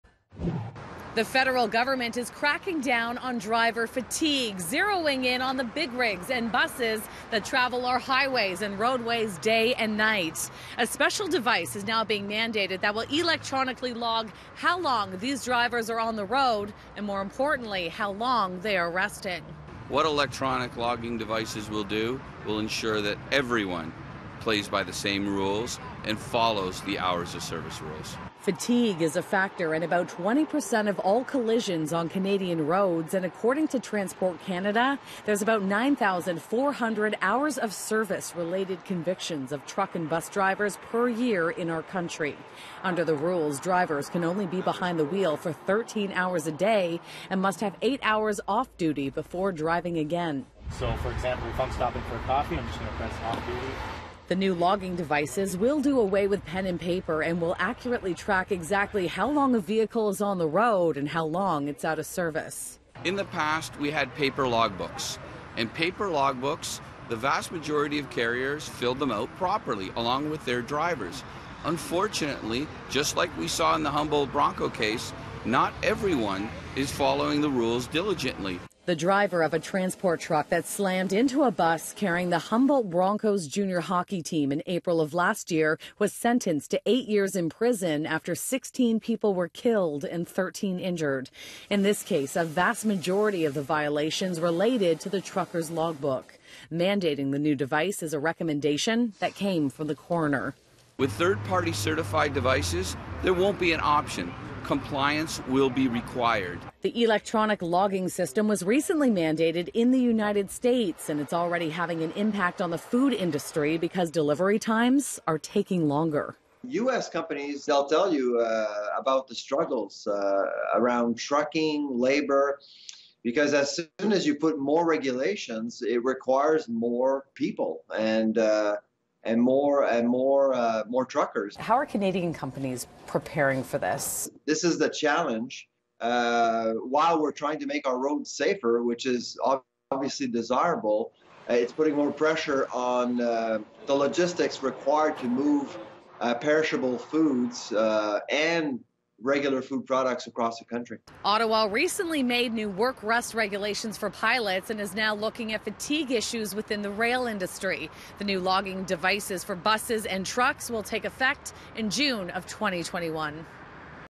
Electronic logbooks for the trucking industry
Electronic logbooks are being introduced to the trucking industry.